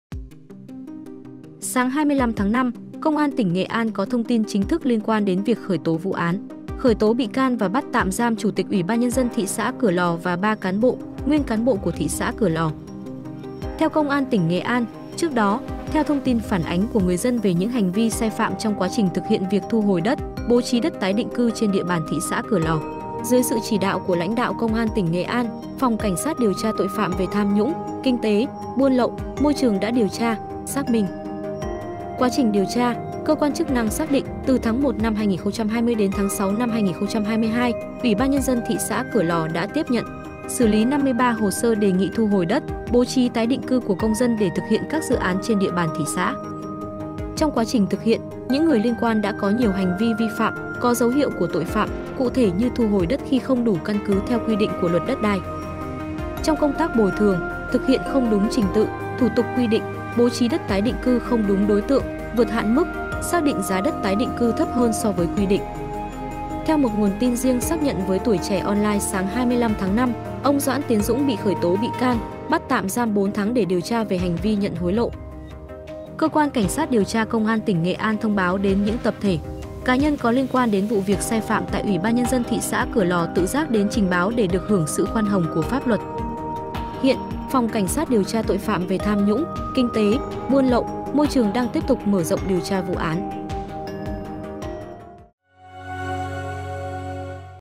Vì sao chủ tịch thị xã Cửa Lò và các thuộc cấp bị bắt?
Nguồn tin riêng xác nhận với Tuổi Trẻ Online ông Doãn Tiến Dũng, chủ tịch UBND thị xã Cửa Lò, bị bắt tạm giam để điều tra về hành vi nhận hối lộ.